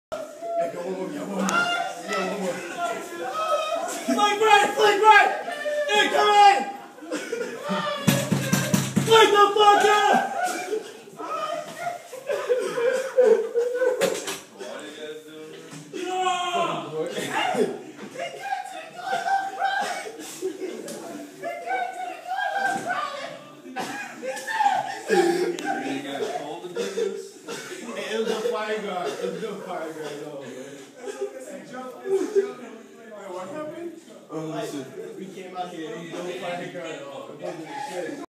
Senior Truck Messing With Baby Truck
This is what happens when you decide to get off fire guard early cuz you think that no one would notice you get woken up by Jr. Truck